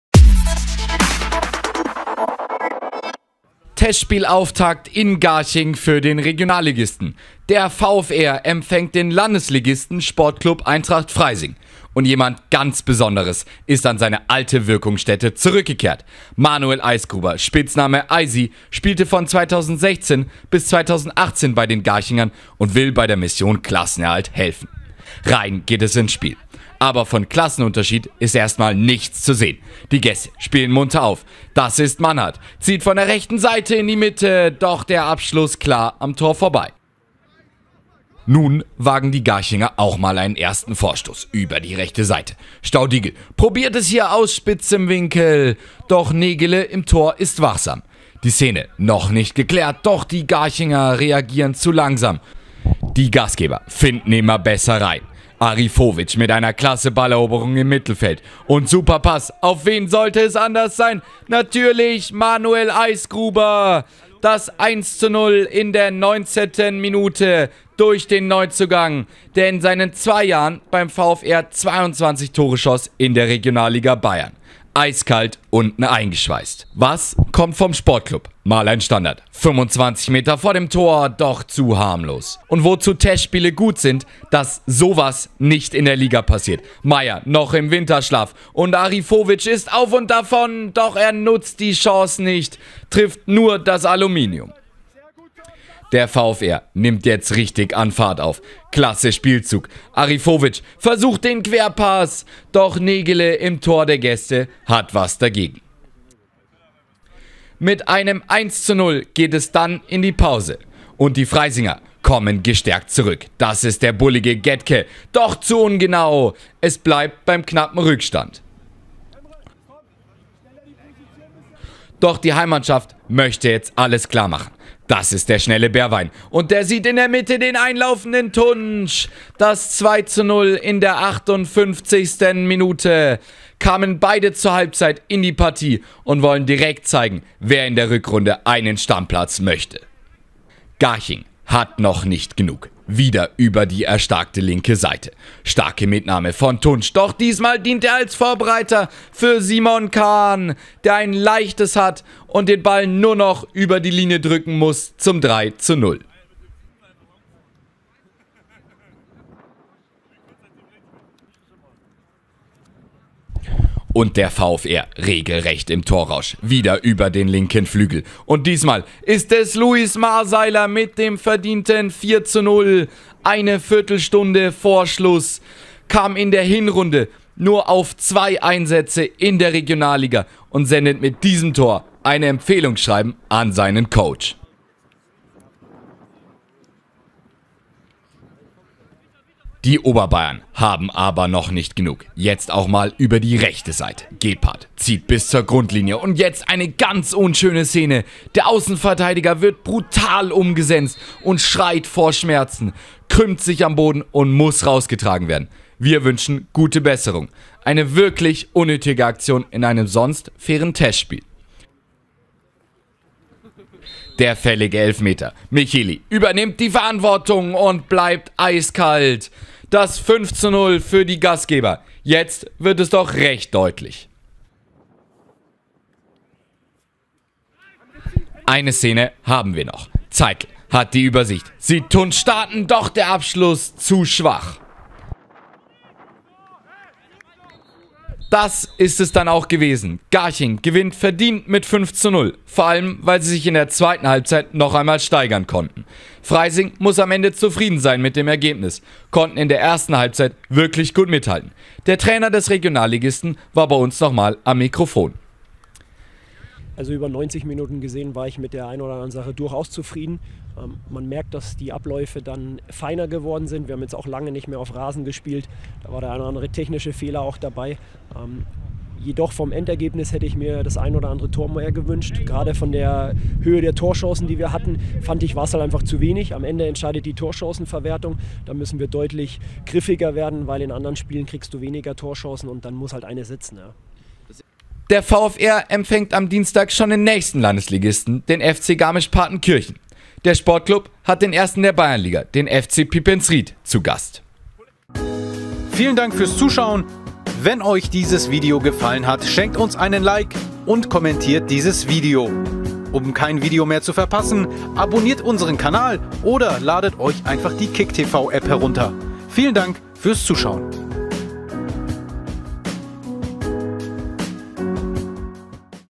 Neuzugang knipst direkt | VfR Garching - SE Freising (Testspiel)
Garching souverän im ersten Test!

Testspiel (01.02.20)

Tore:
1:0 Manuel Eisgruber
2:0 Emre Tunc
3:0 Simon Khan
4:0 Luis Marseiler
5:0 Valentin Micheli

Aufstellung VfR Garching:
24 Joey Brenner (TW), 4 Matthias Strohmaier, 6 Georg Ball, 9 Manuel Eisgruber, 10 Dennis Niebauer, 11 Nikolaos Salassidis, 17 Mike Niebauer, 19 Leopold Krüger, 20 Tom Zimmerschied, 25 Ajlan Arifovic, 27 Mario Staudigl

Einwechselspieler:
22 Sebastian Seibold (ETW), 8 Linus Radau, 13 Emre Tunc, 14 Simon Khan, 15 Ardit Topalaj, 16 Valentin Micheli, 18 Philipp Walter, 21 Mark Zettl, 23 Maximilian Berwein, 29 Sebastian Gebhart, 30 Luis Marseiler

Trainer:
Benjamin Flicker

Aufstellung SE Freising:
1 Patrick Negele (TW), 5 Florian Bittner, 8 Jonas Mayr, 9 Daniel Gaedke, 15 Andi Manhart, 16 Florian Härtel, 17 Osaro Aiteniora, 21 Daniel Zanker, 25 Domagoj Tiric, 26 Nikolai Davydov, 28 Vitus Kirchberger

Einwechselspieler:
4 Sebastian Thalhammer, 7 Florian Machl, 14 Josef Moosholzer, 20 Andreas Schredl, 24 Berin Brzovic

Trainer:
Florian Bittner

Produktion & Kommentar: Jakob Schermer